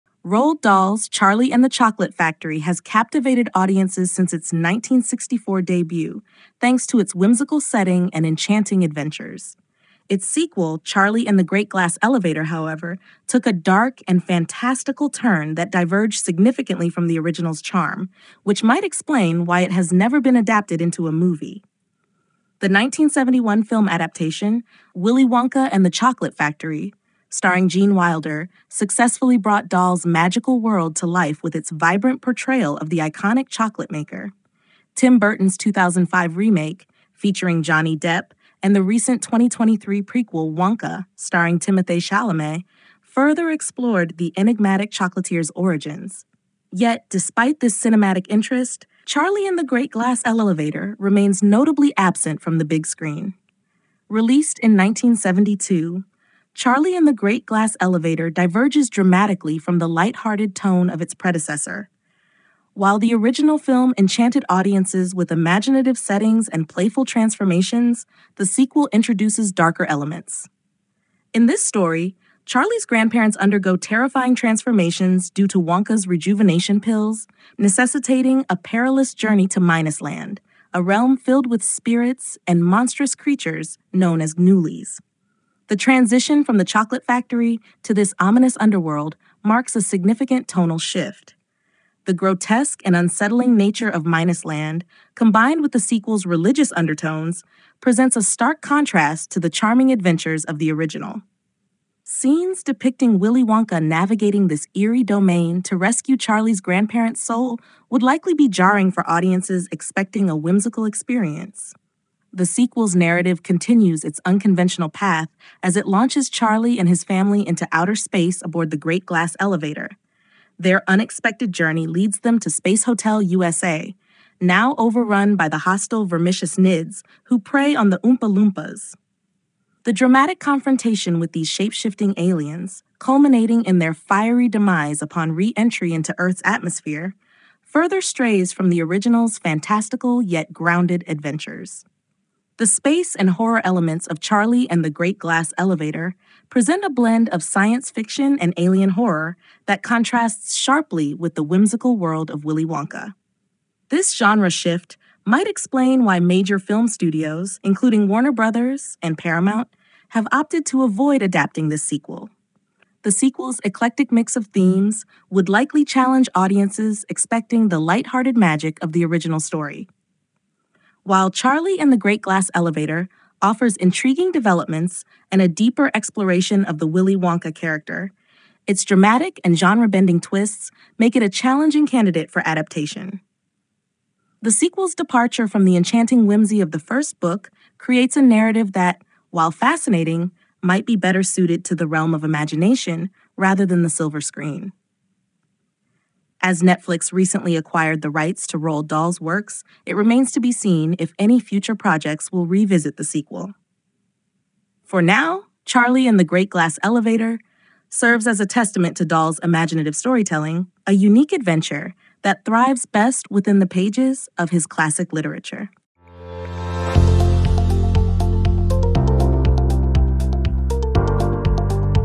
Why Charlie and the Great Glass Elevator Remained Unadapted for the Big Screen
### **Title: Why "Charlie and the Great Glass Elevator" Remained Unadapted for the Big Screen**

Roald Dahl’s *Charlie and the Chocolate Factory* has captivated audiences since its 1964 debut, thanks to its whimsical setting and enchanting adventures. Its sequel, *Charlie and the Great Glass Elevator*, however, took a dark and fantastical turn that diverged significantly from the original's charm, which might explain why it has never been adapted into a movie.

The 1971 film adaptation, *Willy Wonka and the Chocolate Factory*, starring Gene Wilder, successfully brought Dahl’s magical world to life with its vibrant portrayal of the iconic chocolate maker. Tim Burton's 2005 remake, featuring Johnny Depp, and the recent 2023 prequel *Wonka*, starring Timothée Chalamet, further explored the enigmatic chocolatier's origins. Yet, despite this cinematic interest, *Charlie and the Great Glass Elevator* remains notably absent from the big screen.

### **A Sinister Departure from Whimsy**

Released in 1972, *Charlie and the Great Glass Elevator* diverges dramatically from the light-hearted tone of its predecessor. While the original film enchanted audiences with imaginative settings and playful transformations, the sequel introduces darker elements. In this story, Charlie's grandparents undergo terrifying transformations due to Wonka’s rejuvenation pills, necessitating a perilous journey to Minus-land, a realm filled with spirits and monstrous creatures known as Gnoolies.

The transition from the chocolate factory to this ominous underworld marks a significant tonal shift. The grotesque and unsettling nature of Minus-land, combined with the sequel’s religious undertones, presents a stark contrast to the charming adventures of the original. Scenes depicting Willy Wonka navigating this eerie domain to rescue Charlie’s grandparent’s soul would likely be jarring for audiences expecting a whimsical experience.

### **Space Adventures and Alien Horrors**

The sequel’s narrative continues its unconventional path as it launches Charlie and his family into outer space aboard the Great Glass Elevator. Their unexpected journey leads them to Space Hotel U.S.A., now overrun by the hostile Vermicious Knids, who prey on the Oompa Loompas. The dramatic confrontation with these shape-shifting aliens, culminating in their fiery demise upon re-entry into Earth’s atmosphere, further strays from the original's fantastical yet grounded adventures.

The space and horror elements of *Charlie and the Great Glass Elevator* present a blend of science fiction and alien horror that contrasts sharply with the whimsical world of Willy Wonka. This genre shift might explain why major film studios, including Warner Brothers and Paramount, have opted to avoid adapting this sequel. The sequel’s eclectic mix of themes would likely challenge audiences expecting the light-hearted magic of the original story.

### **A Tale Best Left in the Imagination**

While *Charlie and the Great Glass Elevator* offers intriguing developments and a deeper exploration of the Willy Wonka character, its dramatic and genre-bending twists make it a challenging candidate for adaptation. The sequel’s departure from the enchanting whimsy of the first book creates a narrative that, while fascinating, might be better suited to the realm of imagination rather than the silver screen.

As Netflix recently acquired the rights to Roald Dahl’s works, it remains to be seen if any future projects will revisit the sequel. For now, *Charlie and the Great Glass Elevator* serves as a testament to Dahl’s imaginative storytelling, a unique adventure that thrives best within the pages of his classic literature.